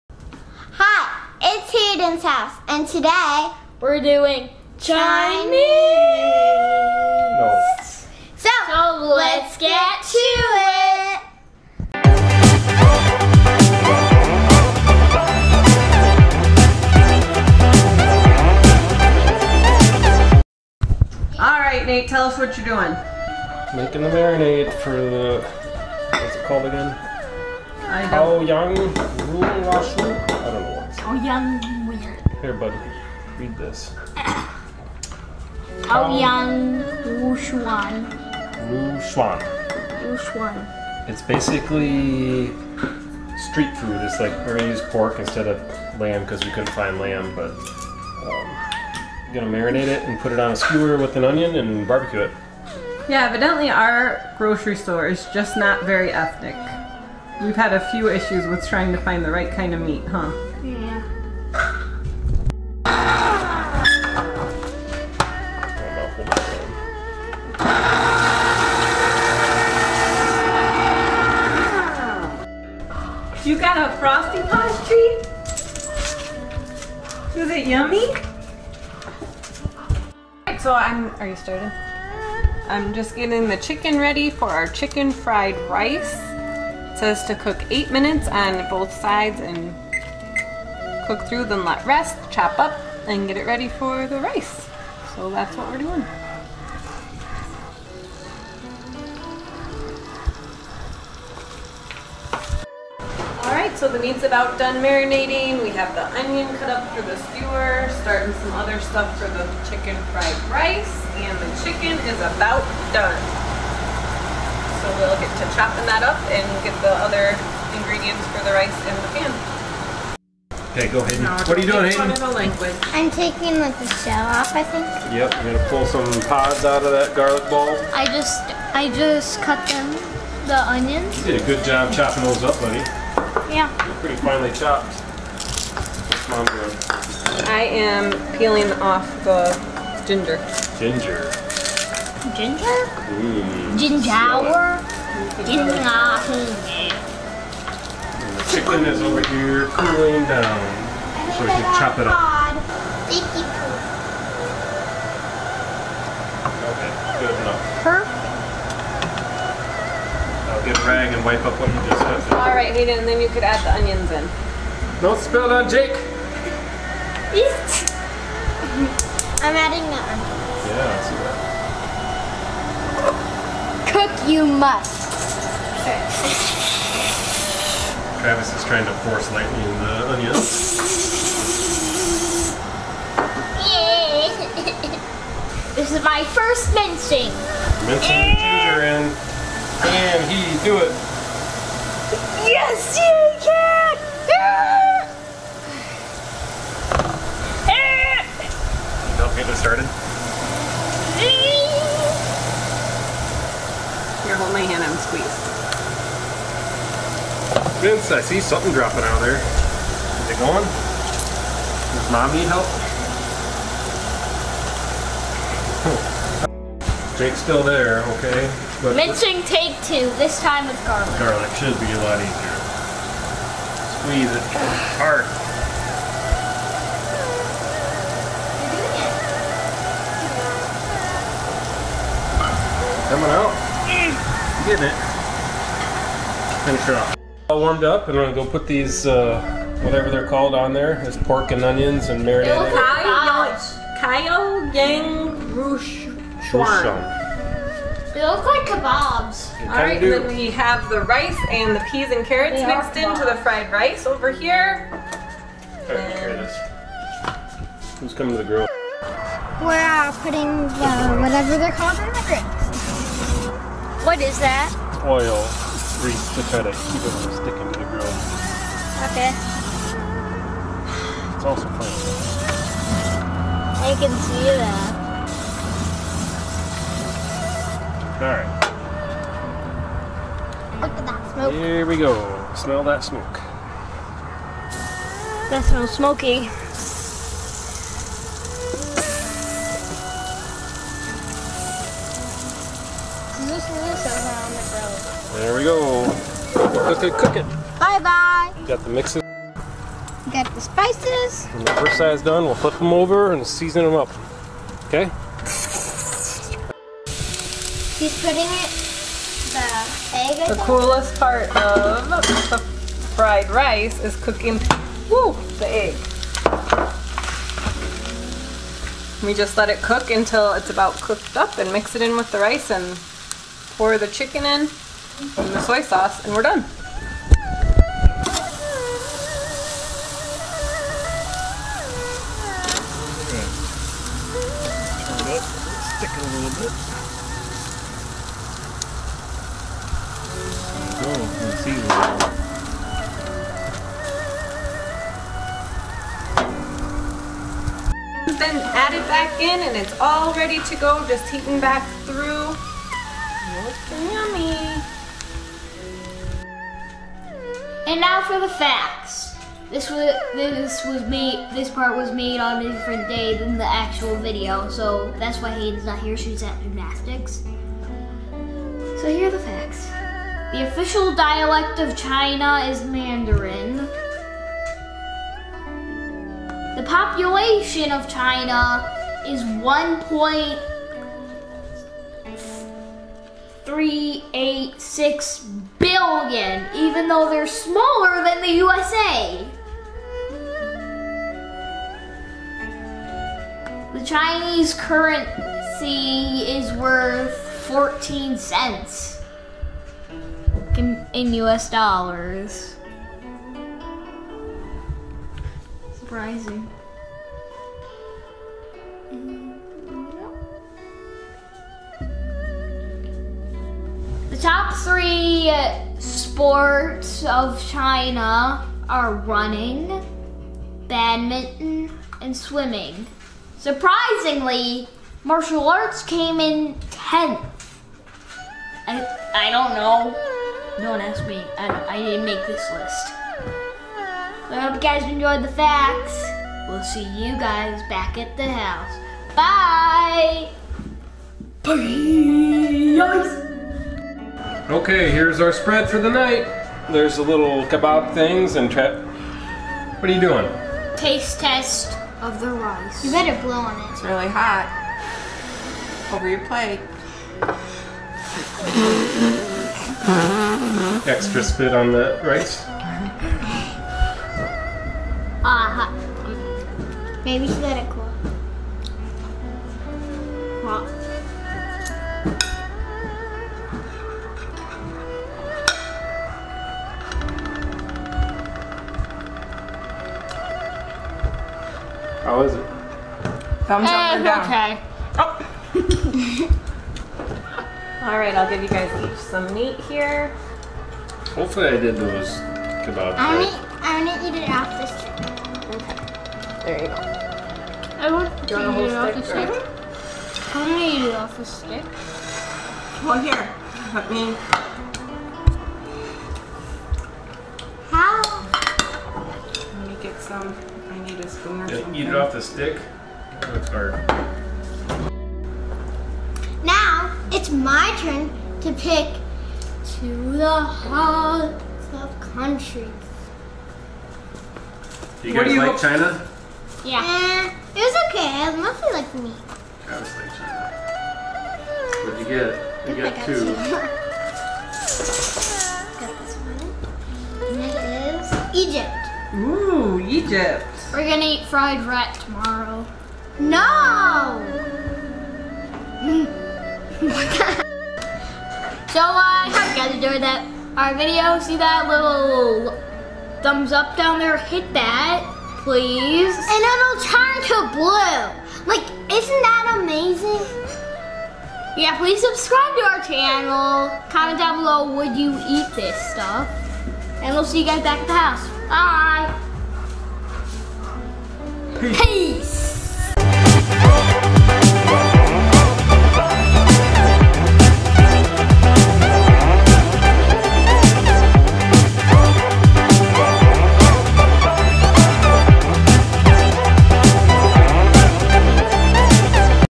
Travel the World with Hayden's House: China
This week we travel to China.  Join us as we try to make something we can't pronounce, but was delicious.  My favorite was the chicken fried rice.  What is your favorite chinese food to order for take out?  The kids love orange chicken and I like chicken egg foo young.

Tammy (Mom), Nate (Dad), Travis, and Hayden.

Hayden's House is a fun family channel.  We feature the kids activities, challenges, vlogs, reviews, games and special projects!